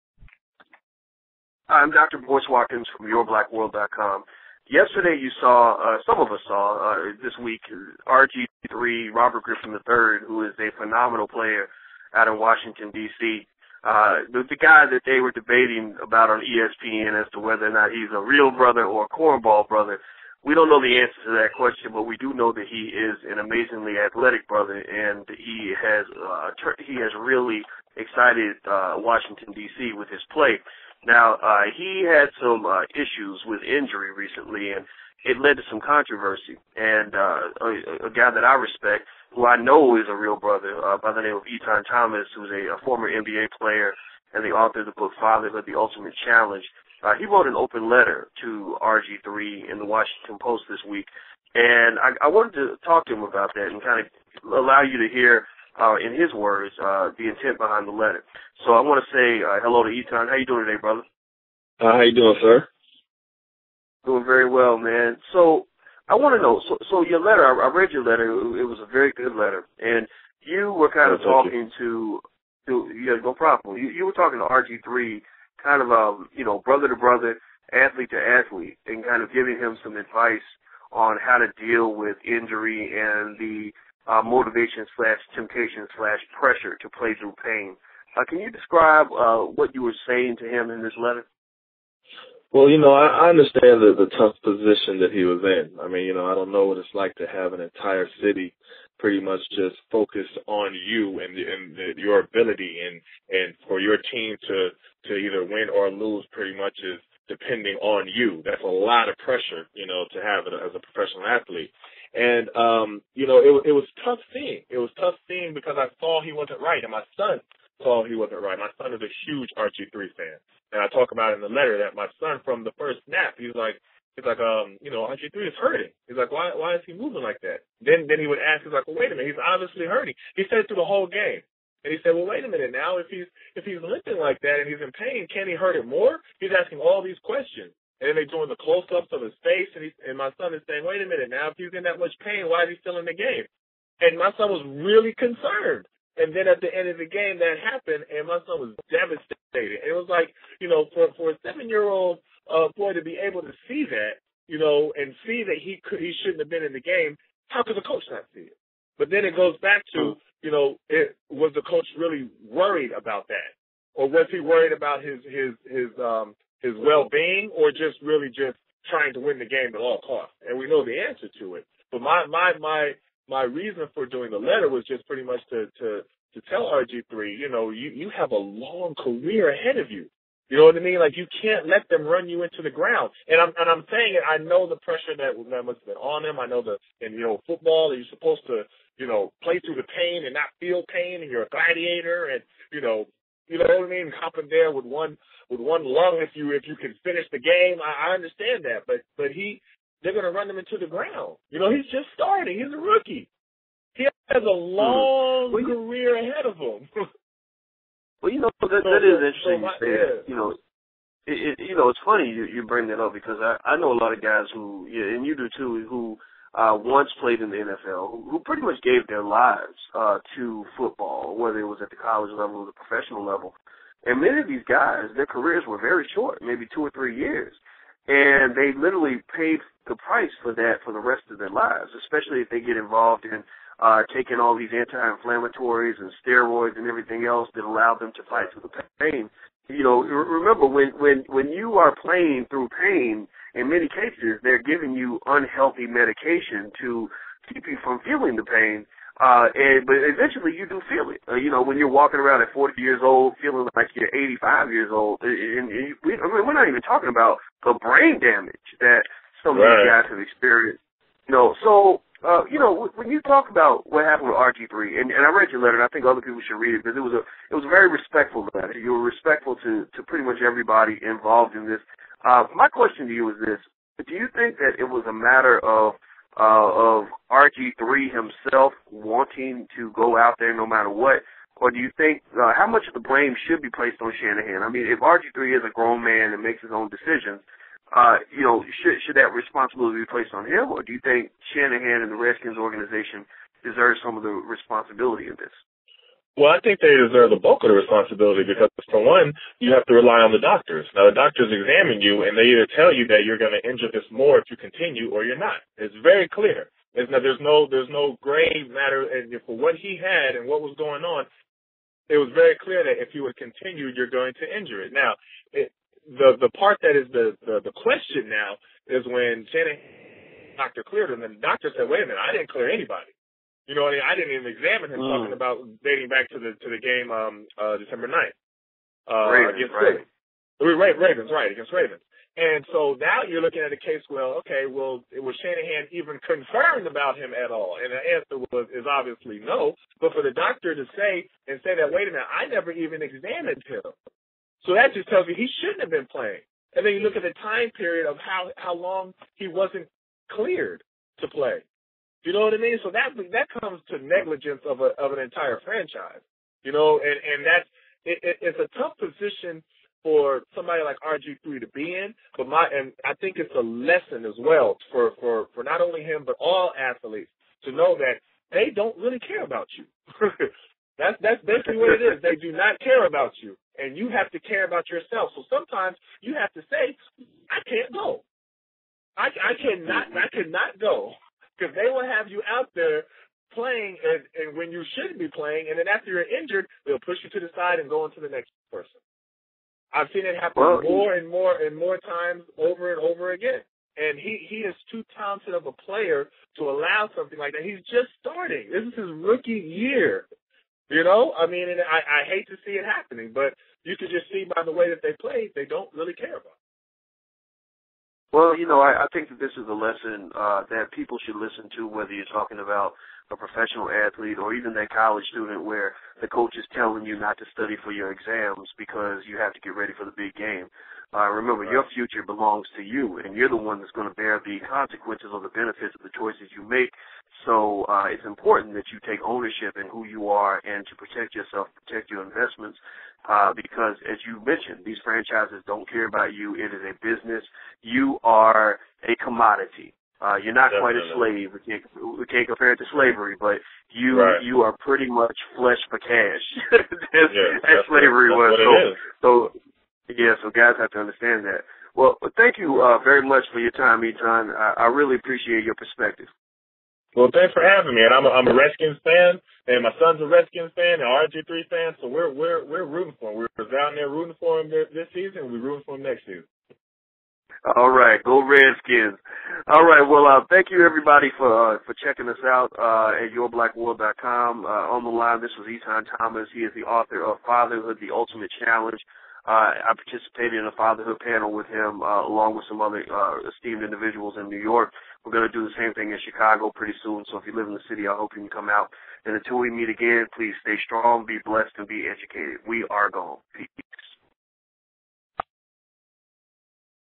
Former NBA Player Etan Thomas Gives Advice to Redskins Quarterback: Don't Play in Serious Pain
Etan Thomas tells Robert Griffin III that he had no business being on the field last week against the Baltimore Ravens.